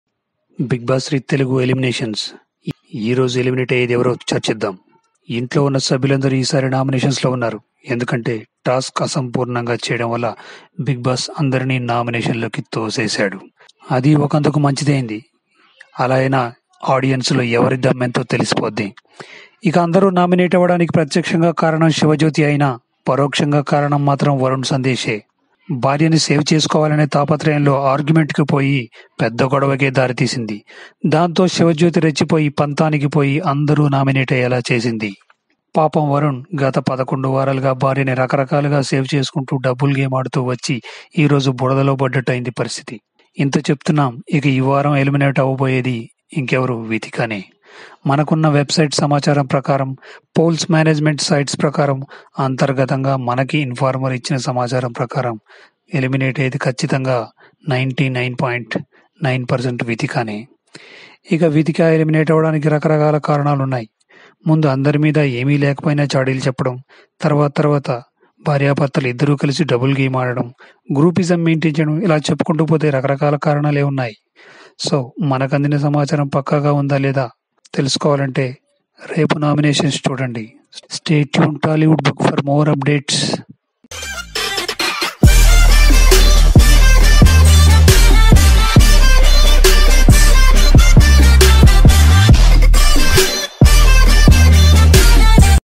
Vithika eliminated l bigg boss 3 telugu today elimination l varun ki dikkevaru
bigg boss season 3 telugu 13th week eliminations lonunchi vithika eliminated...all contestants are in nominations..bigg boss 3 13th week  eliminations are very interesting..srimukhi and Rahul, Baba Bhaskar saved this week till now Saturday..
 bigg boss 3 telugu this week elimination..Varun Sandesh and vithika played well in bigg boss house.